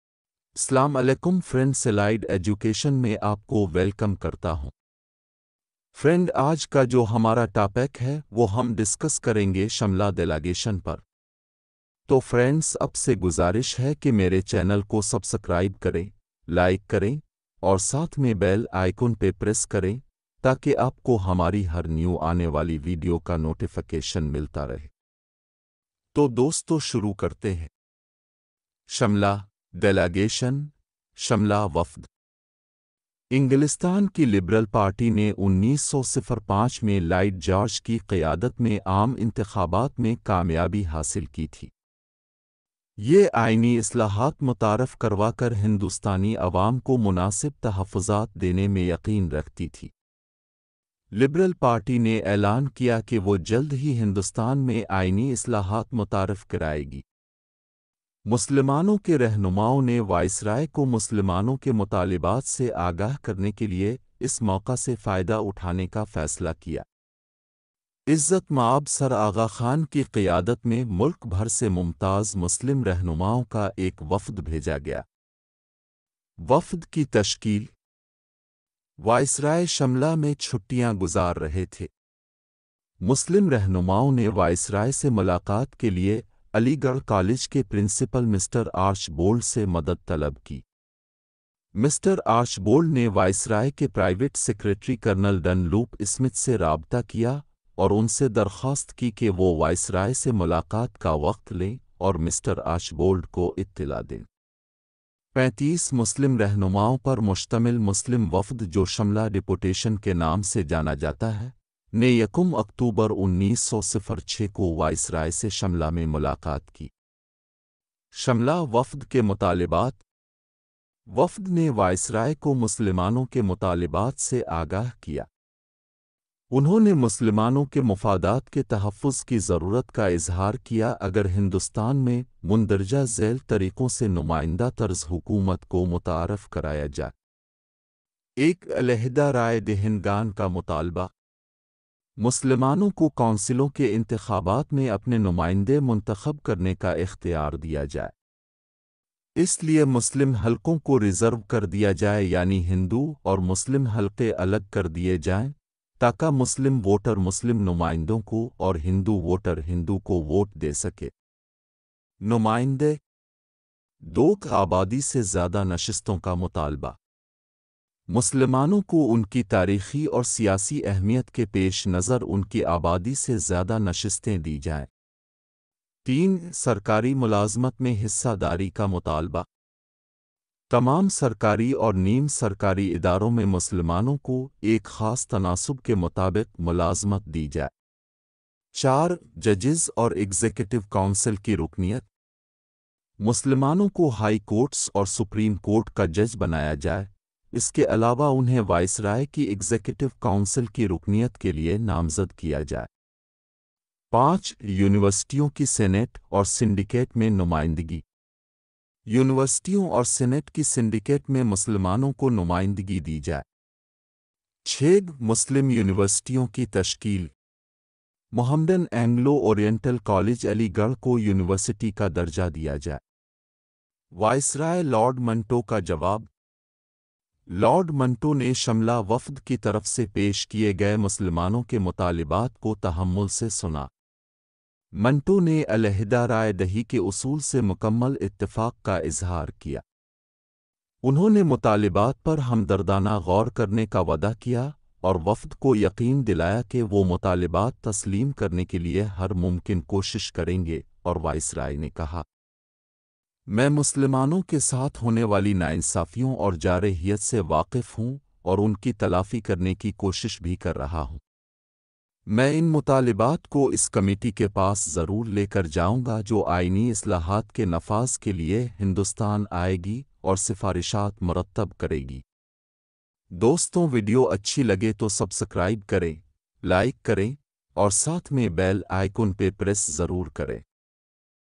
Simla Delegation in Urdu / Hindi | Important | Background | Reasons | Main Points
Simla Delegation.

Liberal Party of England had won the general elections under the leadership of Lloyd George in 1905. It believed in giving adequate safeguards to the Indian people by introducing constitutional reforms. The Liberal Party announced that it would soon introduce constitutional reforms in India. The Muslim leaders decided to avail this opportunity in order to apprise the Viceroy of the Muslims demands. A delegation of prominent Muslim leaders, from all over the country was forriied under the leadership of His Highness Sir Agha Khan.

Formation of the Delegation:

The Viceroy was vacationing at Simla. The Muslim leaders asked for the assistance of Mr. Archbold, the Principal of Aligarh College, to get an appointment with the viceroy. Mr. Archbold got in touch with the Private Secretary of the Viceroy Col. Dunlop Smith and requested him to get an appointment with the Viceroy and nform Mr. Archbold. The Muslim delegation comprising 35 Muslim leaders known as Simla Deputation, called on the Viceroy on 1st October, 1906 at Simla.

Demands of Simla Delegation:

The deputation apprised the Viceroy of the Muslim demands. They expressed the need for the protection of the Muslim interests if representative form of government was to be introduced in India by the following ways.

1. Demand of Separate Electorate:

The Muslims should be given the authority to elect their representatives in the elections of the councils. For this reason the Muslim constituencies should be reserved meaning the Hindu and Muslim constituencies should be separated, so that Muslim voter could vote for Muslim representatives and Hindu voters for Hindu representatives.

2. Demand for More Seats than Population:

Muslims should be given more seats than their population because of their historical and political importance.

3. Demand for Share in Government Service:

Muslims should be given employment according to a special proportion in all the government & semi-government organizations.

4. Membership of Judges and Executive Council:

Muslims should be made judges of High courts and Supreme Courts, besides they should be nominated for the membership in executive council of the Viceroy.

5. Representation in the Senate and Syndicate of Universities:

Muslims should be given representation in the syndicate of the universities and senate.

6. Making of Muslim Universities:

Mohammedan Anglo Oriental College Aligarh should be given the status of a University.

Response of Viceroy Lord Minto:
Lord Minto gave a patient hearing to the demands of the Muslims presented by the simla deputation. Minto expressed his complete agreement with the principle of separate electorate. He promised to give sympathetic consideration to the demands and assured the deputation that he would do all what was possible to accept the demands and Viceroy said:-

I am aware of the injustices and aggression inflicted upon the Muslim and I am also trying to compensate them. I will definitely take these demands to the committee which will come to India to implement constitutional reforms and will compile the recommendations.